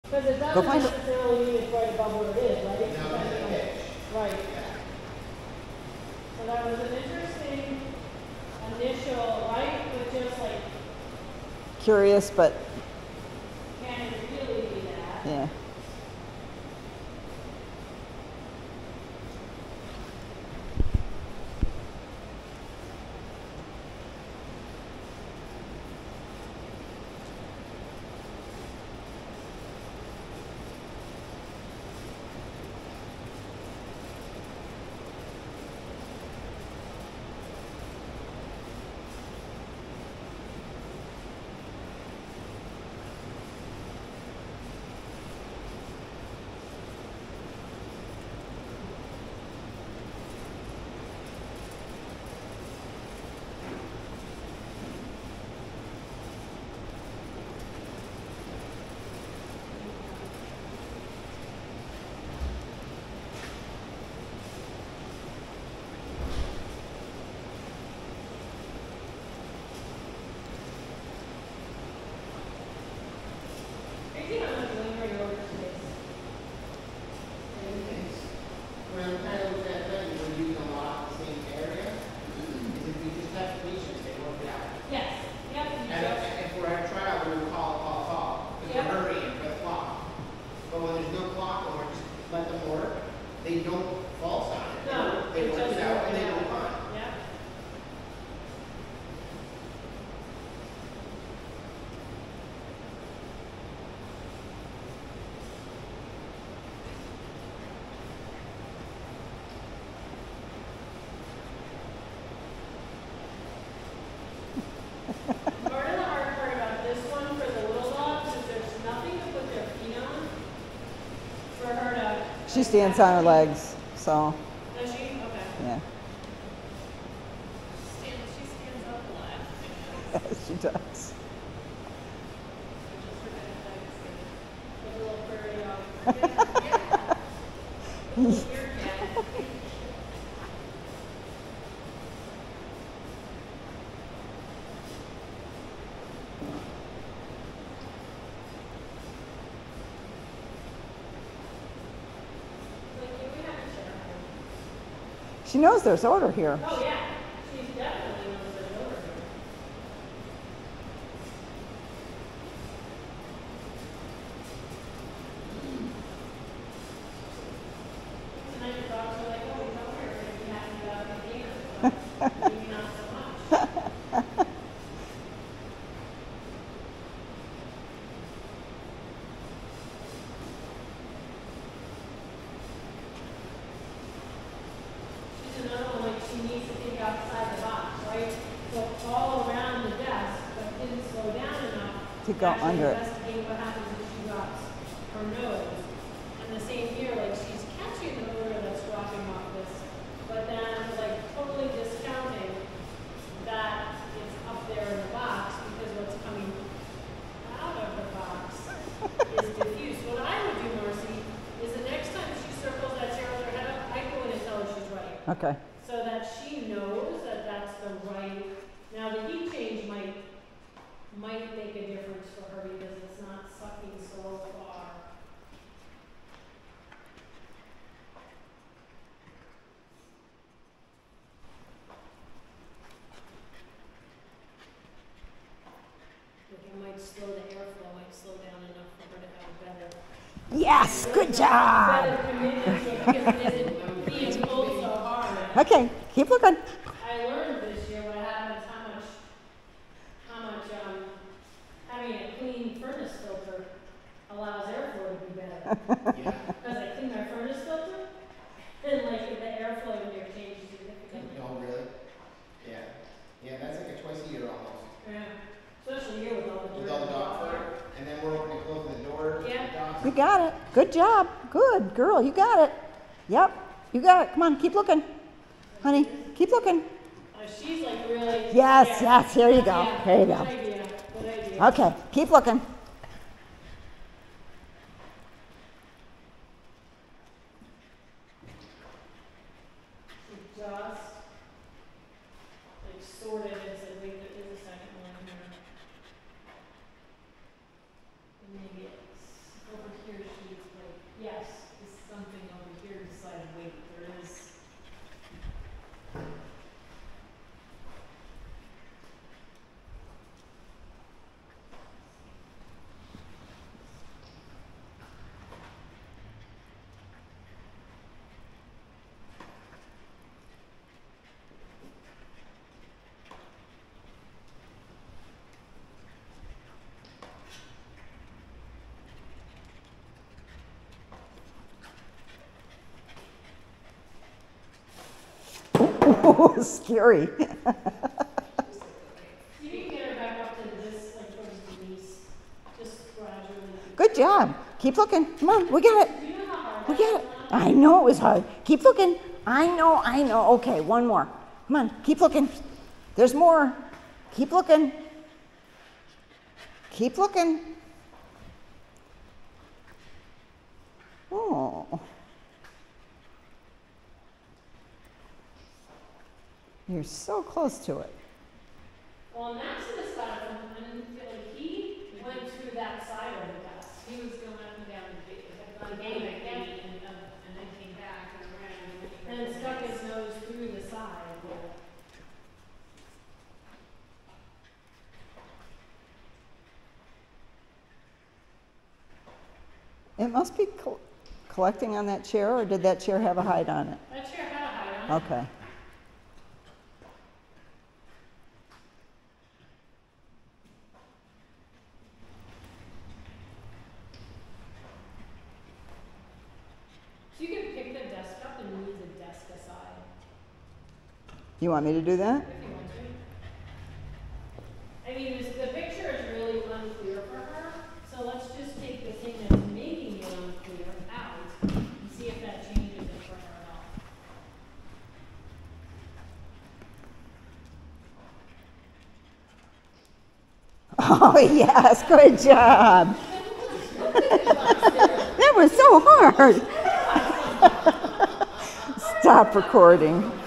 scent work class 3/6/2024 3rd search
This was our third search in class for the night. There were two hides - Honey found the elevated hide fairly quick but didn't commit on it right away. The second hide was on the floor under the play desk and she didn't seem to be getting enough information to locate it until I moved the play desk.